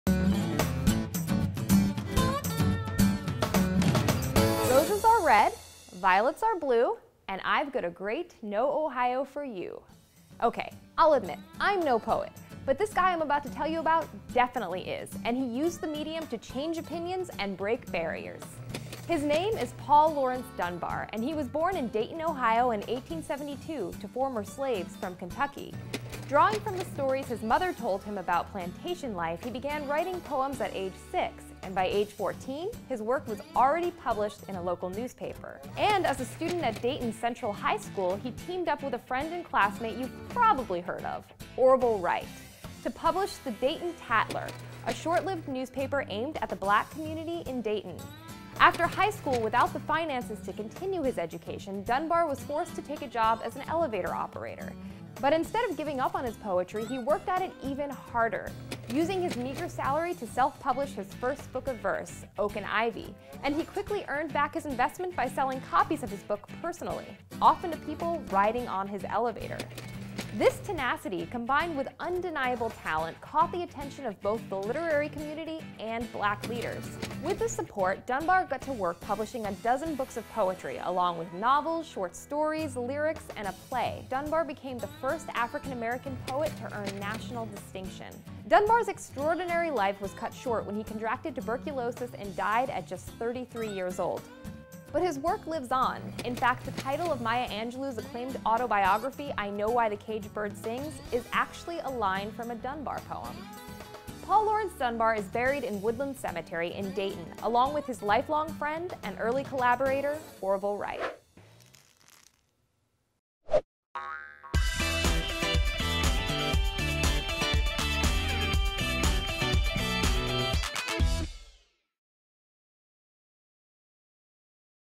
Paul Laurence Dunbar, African American Poet | Know Ohio
The famous playwright and poet was from Ohio! Mary shares his story.

NewsDepth is production of WVIZ PBS ideastream.